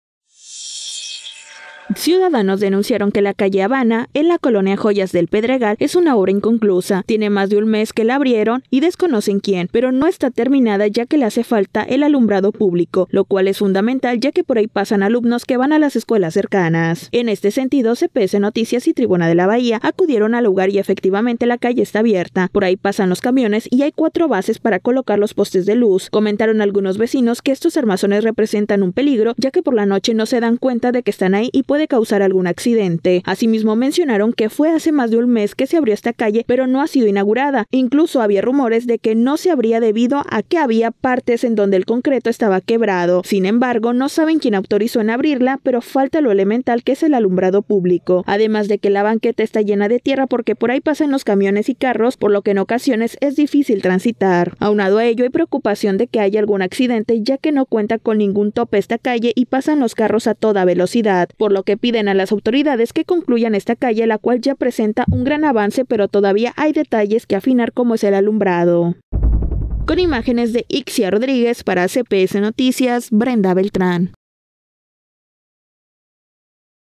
Denuncian vecinos calle Habana inconclusa | CPS Noticias Puerto Vallarta
Ciudadanos denunciaron que la calle Habana en la colonia Joyas del Pedregal, es una obra inconclusa, ya que le hace falta el alumbrado público.